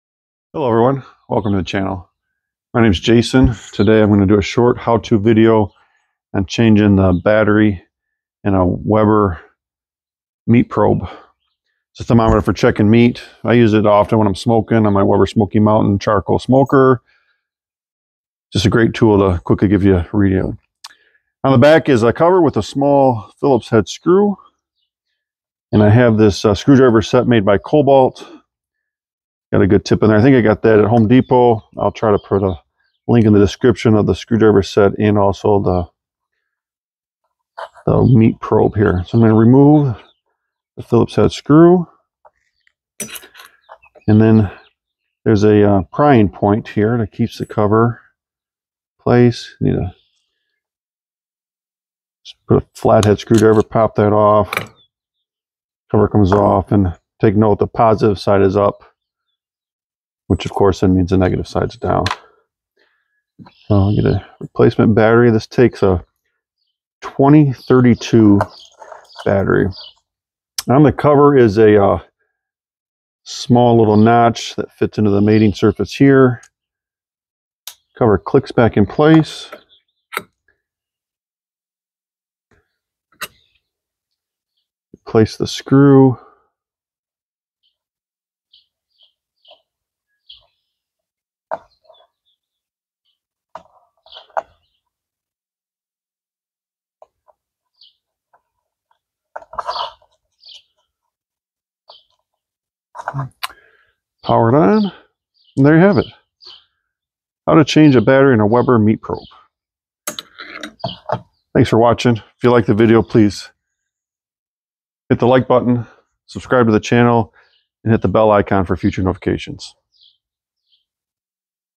Weber Meat Probe Battery Change
How I change the battery in my Weber meat probe thermometer.